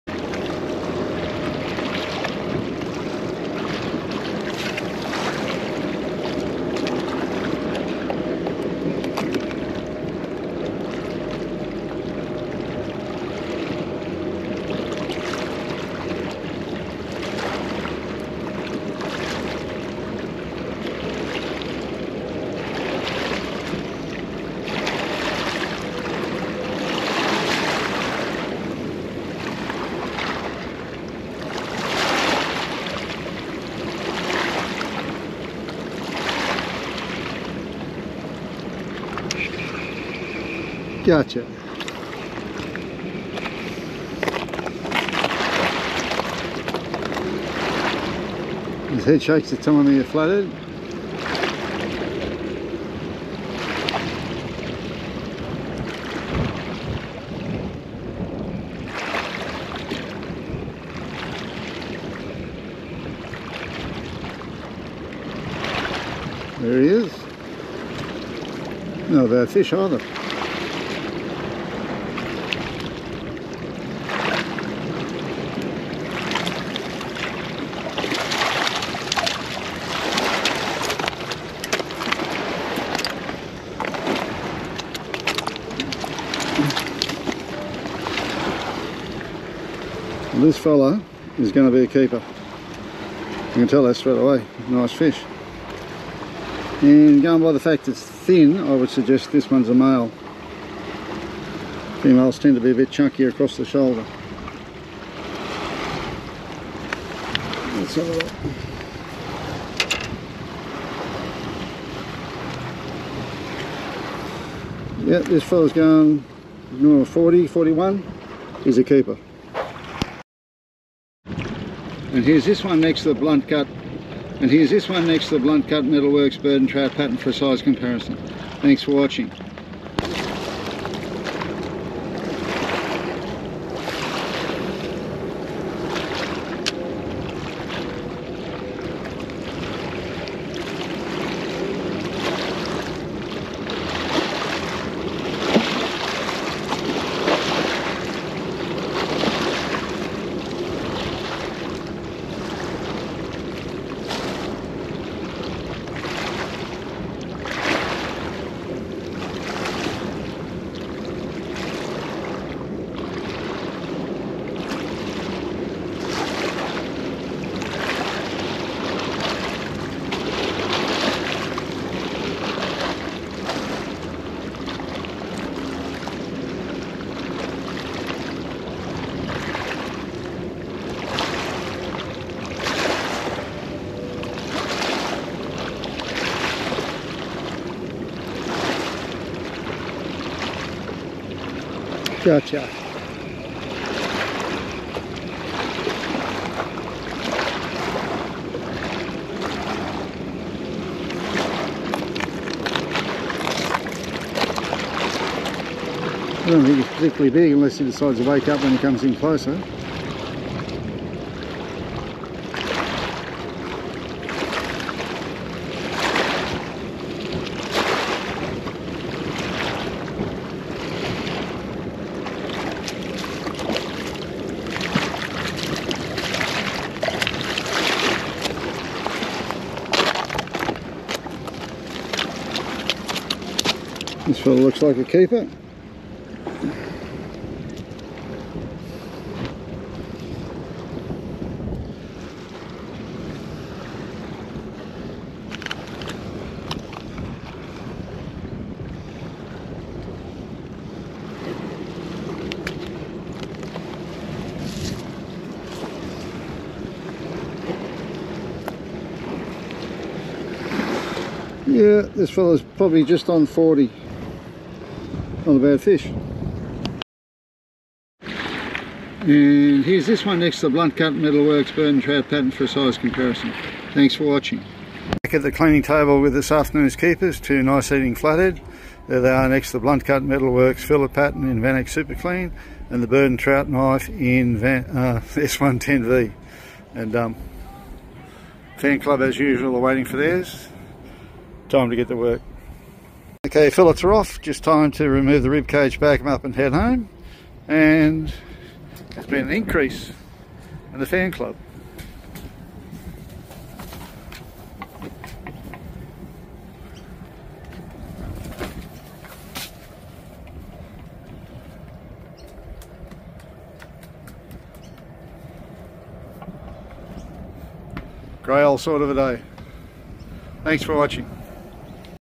A pair of Flathead caught with different lures.
Another windy trip that required choosing lures that cast well into the wind. First Flathead was caught with a TT Quake Power Vibe 60mm in yabby colour, with the second one taking a Samarki Vibelicious Thumper Tail 70mm in Pink Lady UV colour.